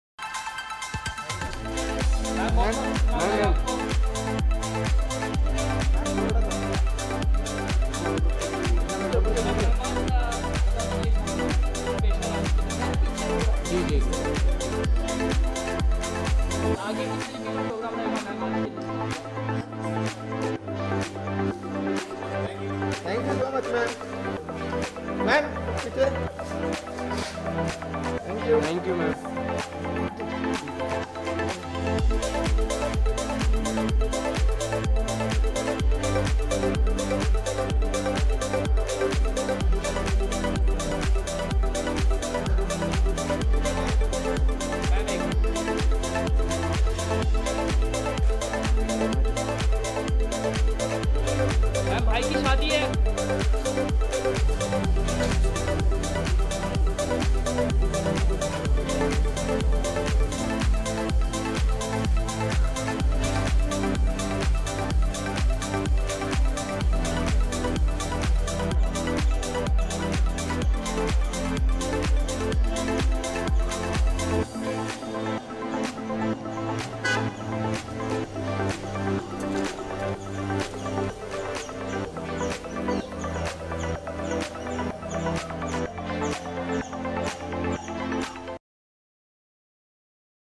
रणबीर-आलिया की शादी
करण जौहर से करीना, करिश्मा रणबीर की वेडिंग स्टार सेरेमनी। मां नीतू कपूर और बहन रिद्धिमा ने सभी का मनोरंजन किया। करीना से लेकर करण जौहर तक, करिश्मा रणबीर की शादी में पहुंचे सितारे। मां नीतू कपूर और बहन रिद्धिमा ने सभी का स्वागत किया.
The News Abtak